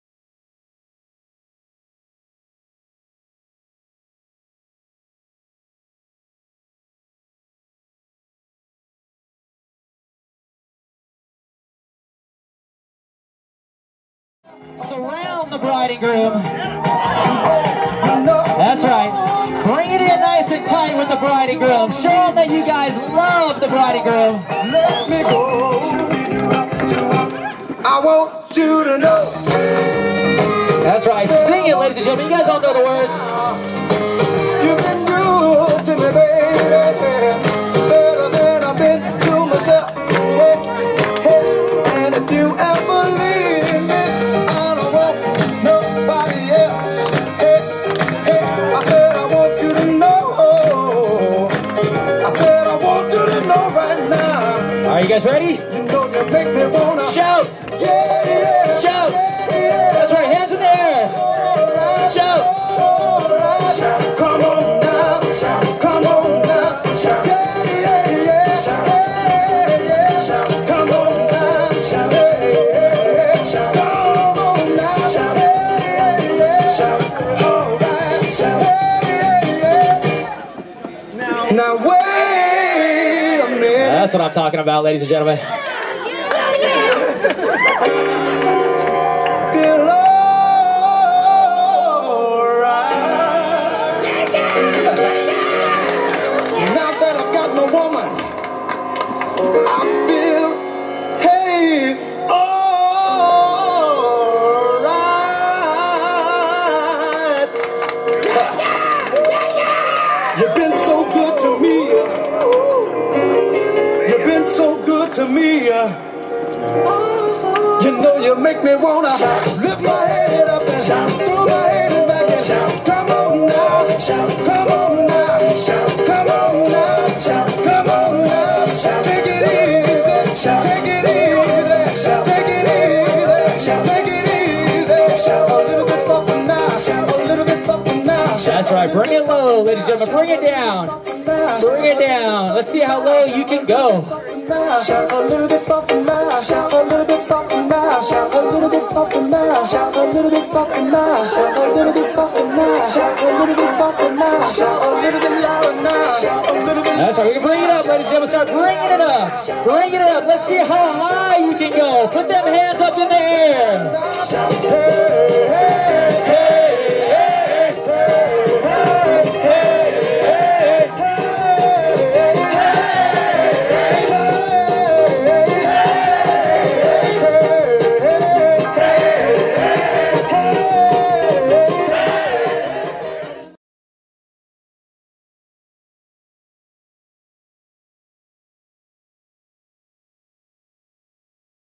Elite Event DJ services Wedding dance April 2016
Mr and Mrs Drew & Stephanie Kinnard. April 16th 2016. Foothills Golf Club, Phoenix Arizona. DJ Elite and DJ Cupcake bringing out all of the guests for a good time with the new bride and groom